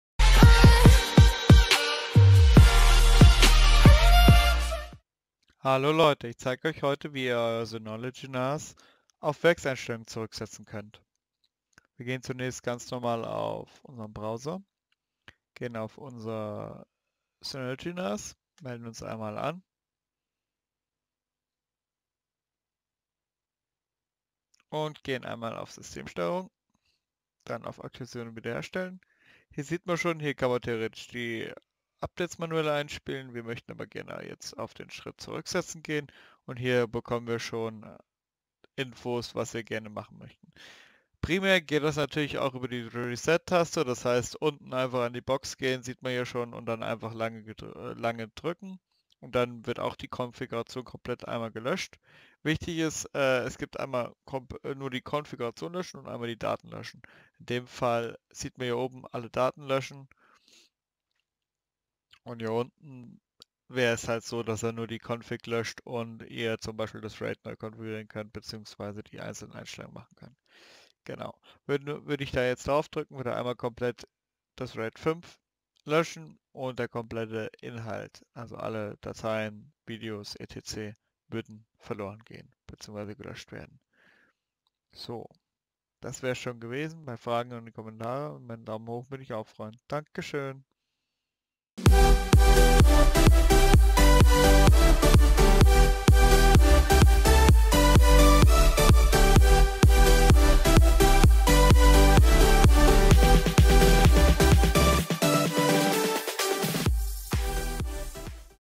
Diskstation auf Werkseinstellungen zurücksetzen | Marcel Scholz
Hallo Leute,

ich zeige euch heute wie ihr eure Diskstation auf Werkseinstellungen zurücksetzen könnt.

MARCEL SCHOLZ